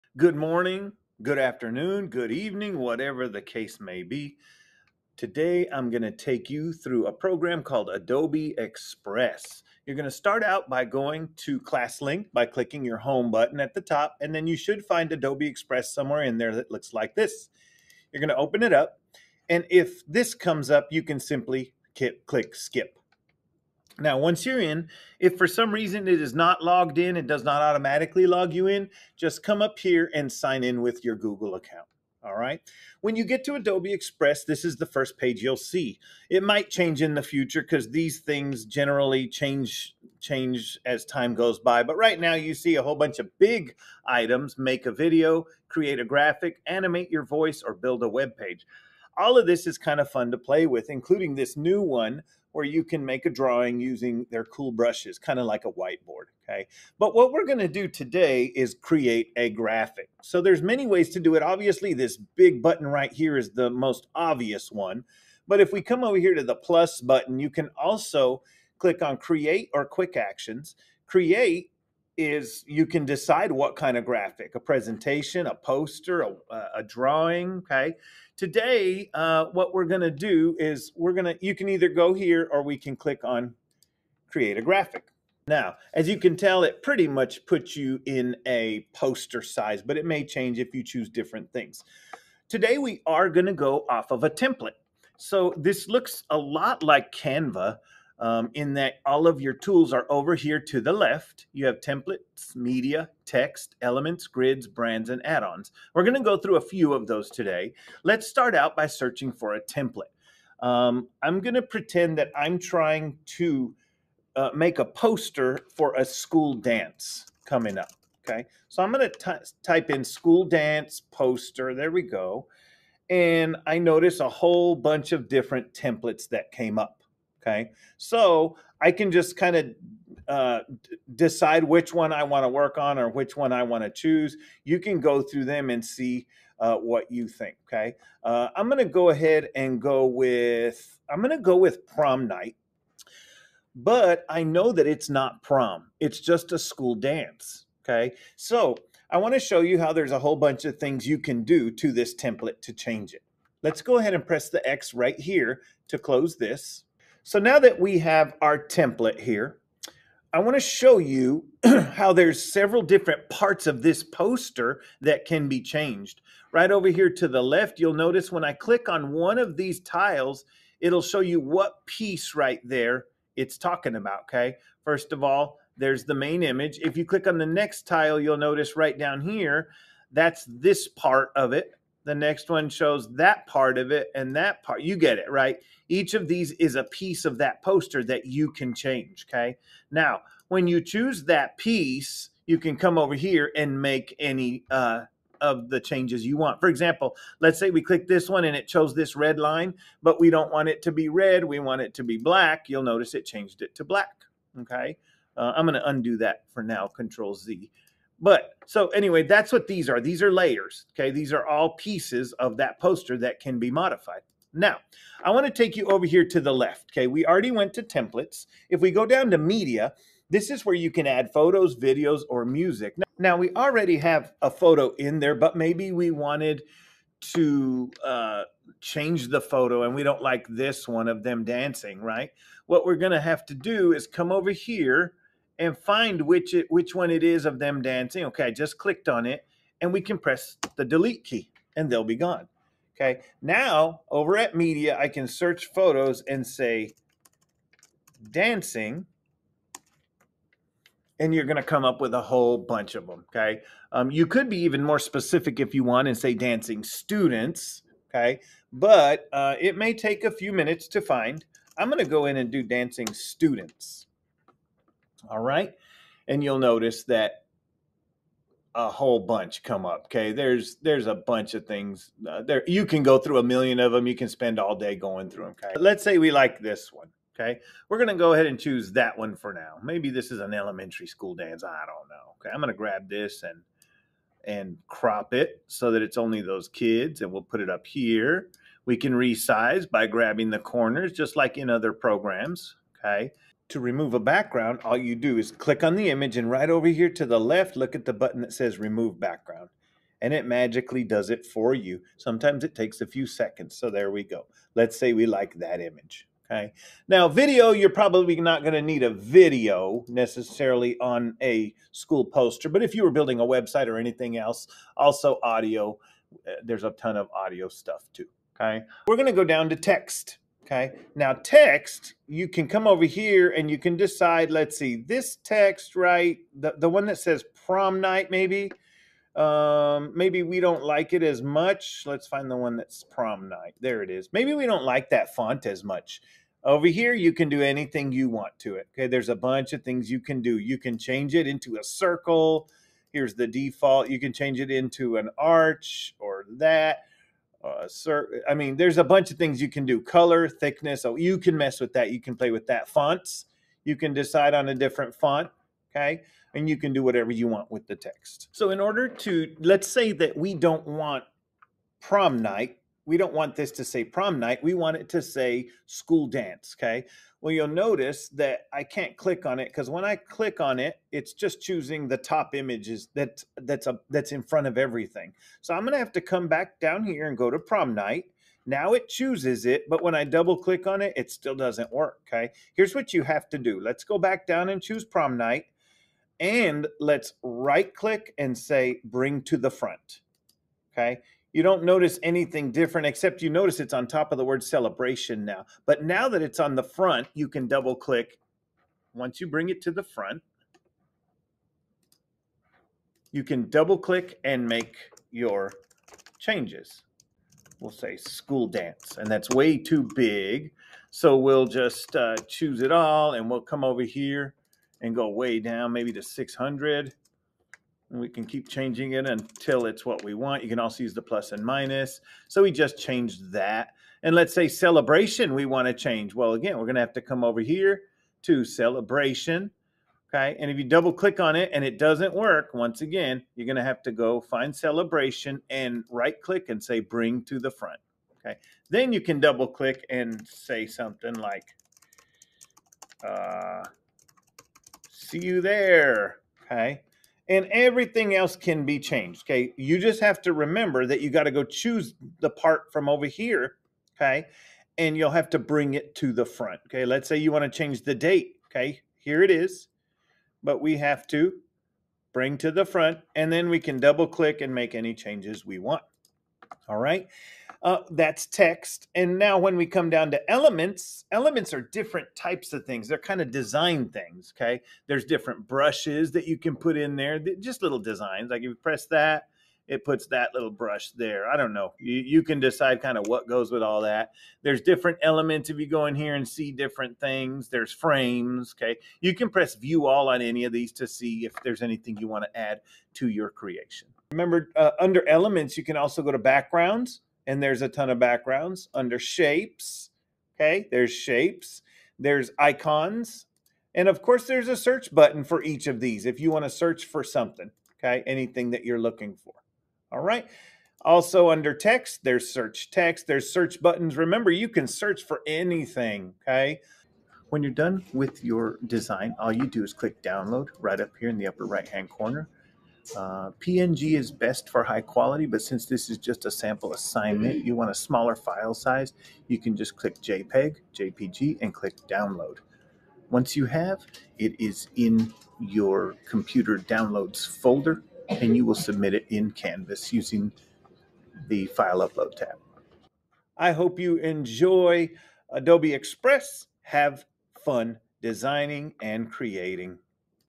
Adobe Express Image Creation Basics
0:00 Create a Graphic
1:32 Choose a Template
2:37 Layers
3:34 Photos
4:53 Remove Background
5:19 Text
7:49 Elements
8:37 Save/Download Design